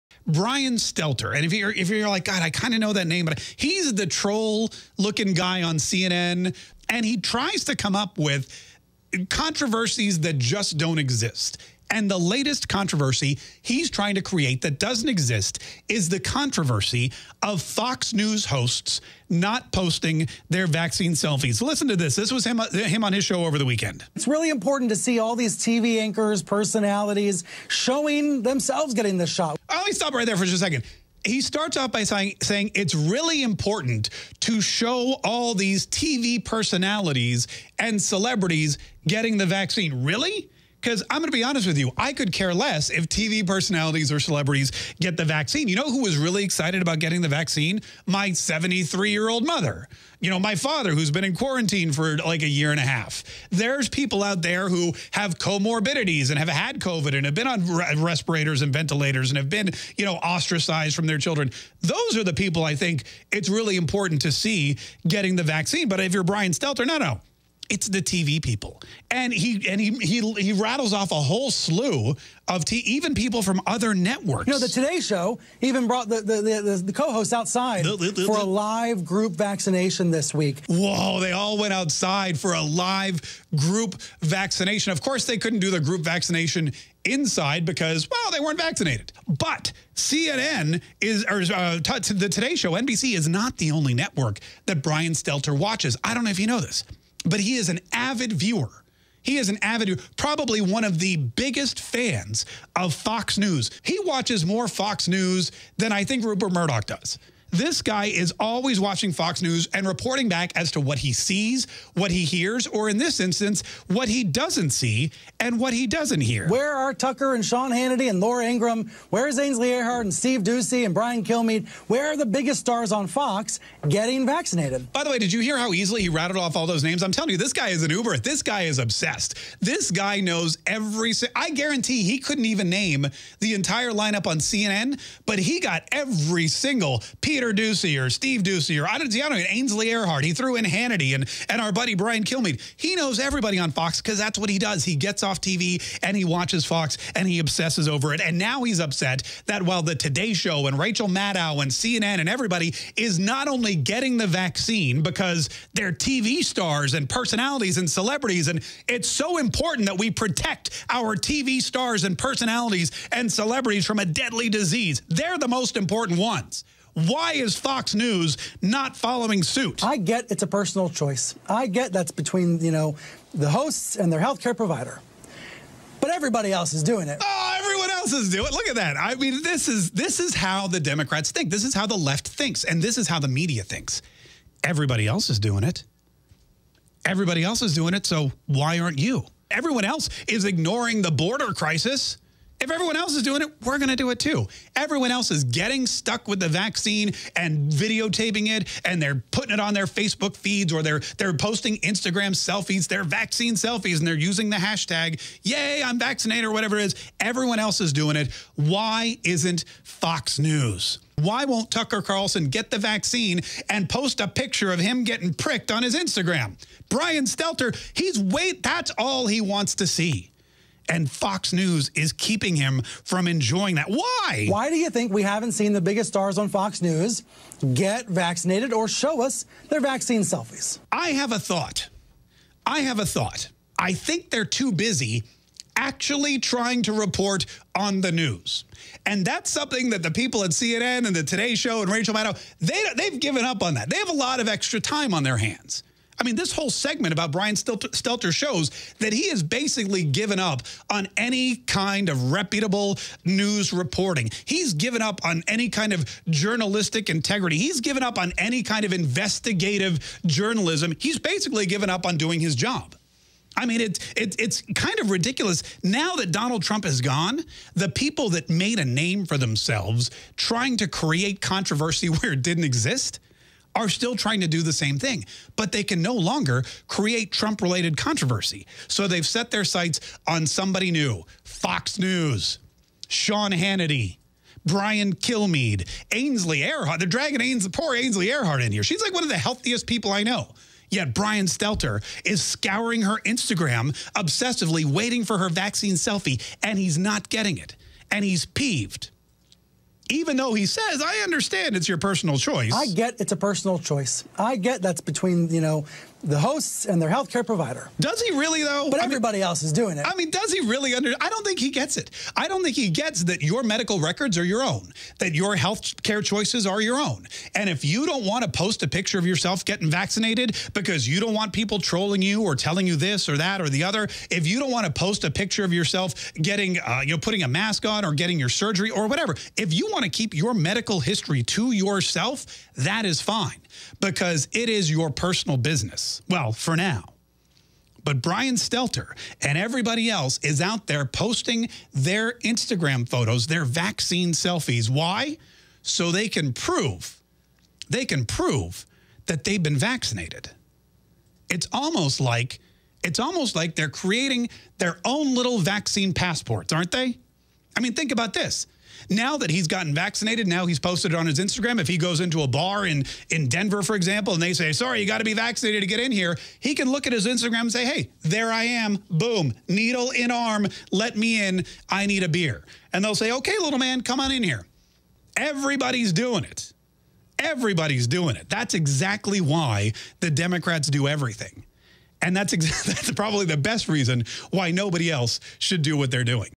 CNN'S STELTER FURIOUS AT FOX FOR THE DUMBEST REASON!!
Keeping your medical history to yourself will be a thing of the past!
YouTube DEMONETIZED Us! But we still have...
Support Free Speech!!!